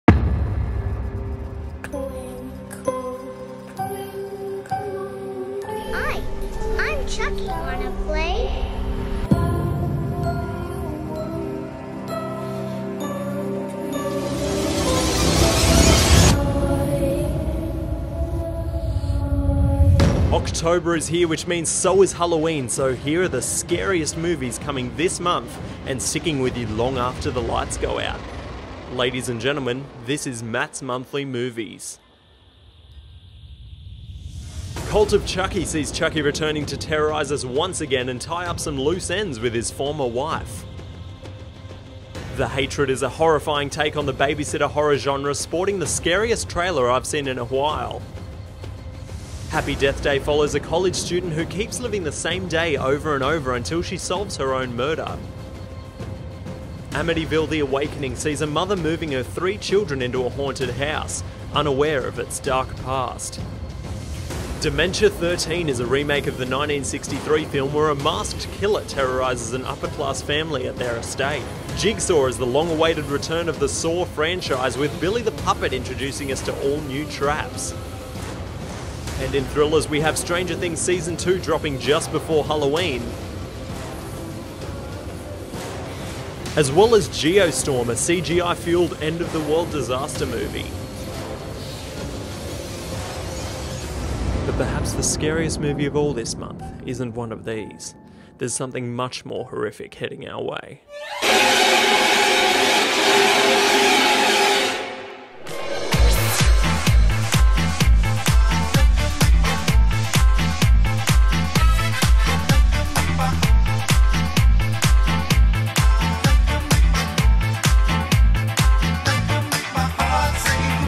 UPCOMING HORROR MOVIES FOR HALLOWEEN 2017
Check out the scariest movie experiences coming to your big screens this month in "Matt's Monthly Movies."